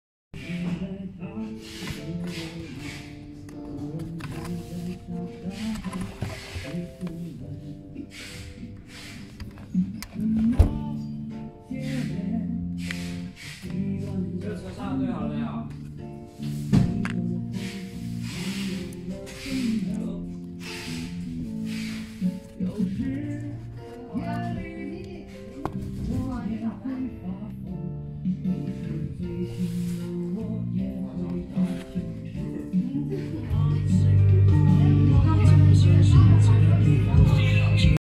Witson Carplay connection for KS6838
ONLY the black USB cable with "安卓6P USB线“ words on it is for Carplay connection.
if you don't connect your iphone to this USB port, Carplay won't work on this KS6838 stereo.